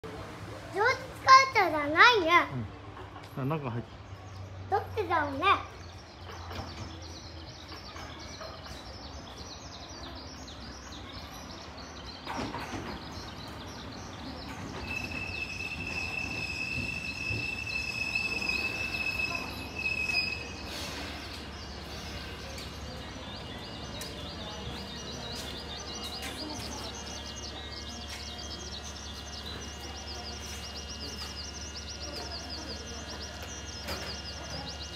Local train series #7500 of Chichibu Railway, coming to Fukaya Hanazono station
7507F has been operated as a wrapping vehicle "coloring combined" since November 2, 2019
In order to replace the aging 1000 series trains, three cars (1st, 4th, and 5th cars) of the Tokyu 8090 series 5-car train set that was used on the Tokyu Oimachi Line were transferred, and when operated on the Chichibu Railway, a 3-car train set is modified to